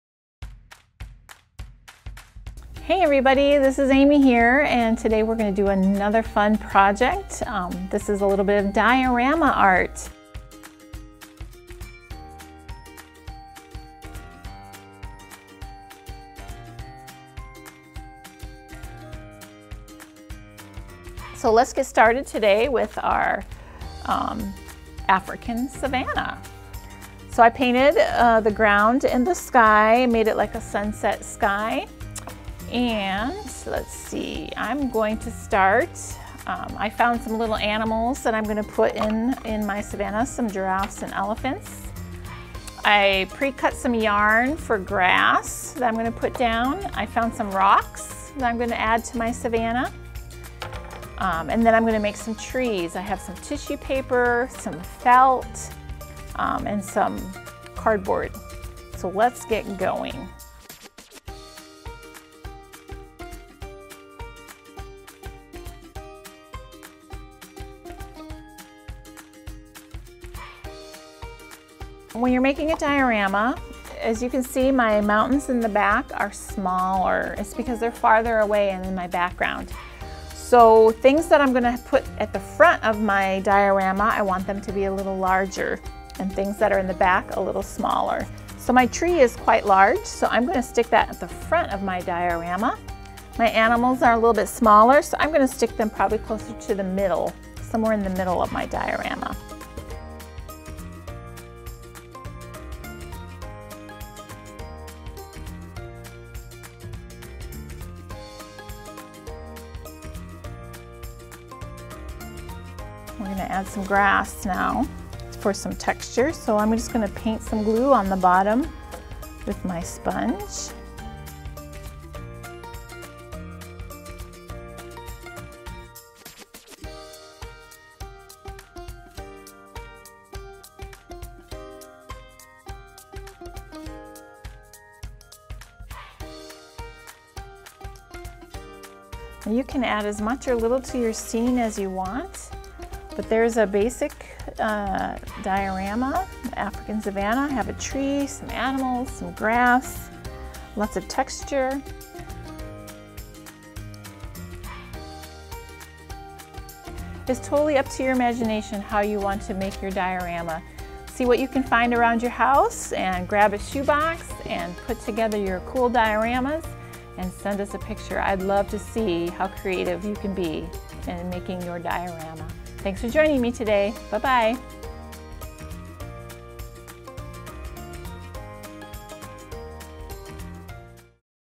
Art Project For Kids: How To Design A Diorama
Ready to use your imagination to bring a whole new world to life? 🌍 Then check out this art project!

In this video, Amy shows you how to design Diorama Art.

Dioramas are a great way to bring landscapes, stories, and events to life - all while using items from around your house! In this video, Amy creates an African Safari as an example. You can create any diorama you like!

Recommended Materials:
- Shoebox & Lid
- Paper (cardstock, tissue, etc.)
- Markers
- Paints
- Fabric / Felt
- Hot Glue Gun
- Duct tape (to tape the shoebox & lid together)
- Any other items from your home or yard (use your imagination!)

When you’re done creating your diorama art, we’d love to see it!

An Academic Success video from the Boys & Girls Clubs of Benton and Franklin Counties Virtual Club.